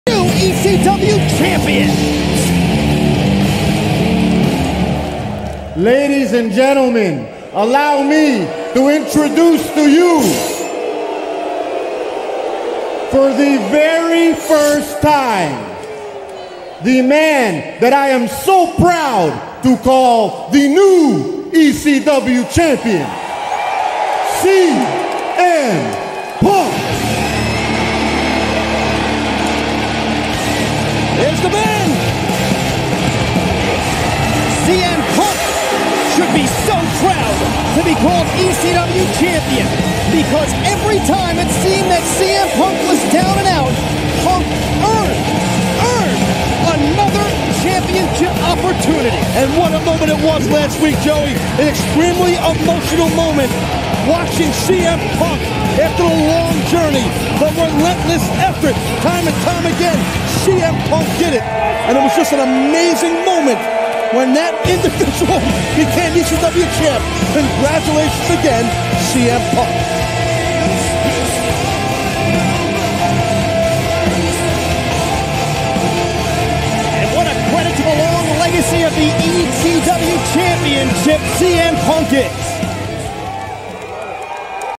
CM Punk's First Championship Entrance In WWE (ECW Champion)
CM Punk makes his first entrance as a champion in WWE, one week after beating John Morrison to capture the ECW Championship